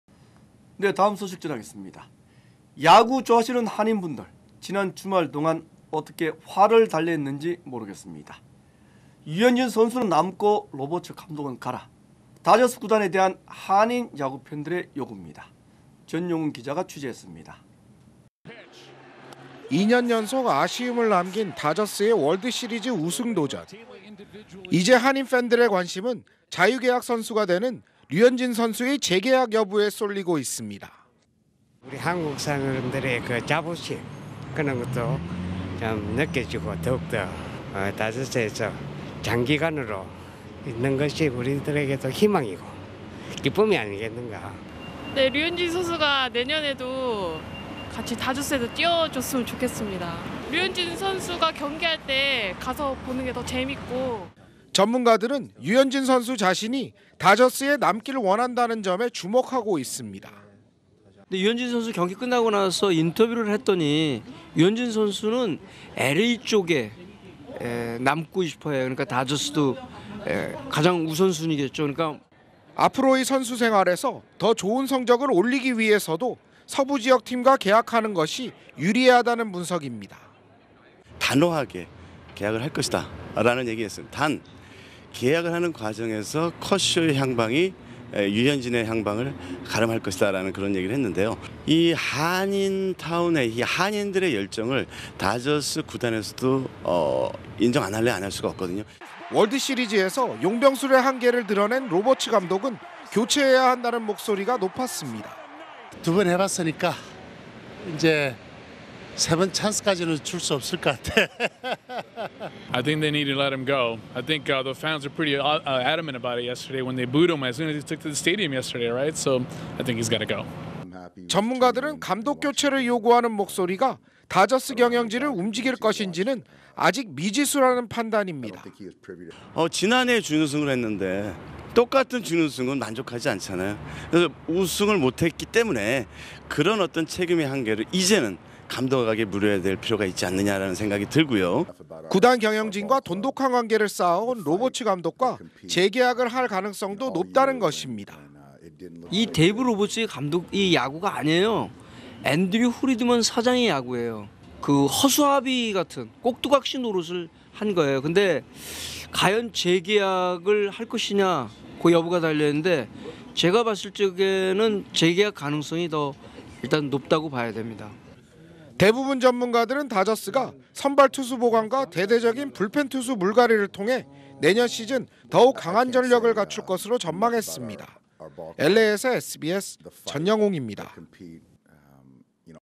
한인팬들 "류현진 남고..로버츠는 가라"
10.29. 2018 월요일
류현진선수의 다저스 재계약 여부에 관심이 집중되고 있습니다
취재: 전영웅 기자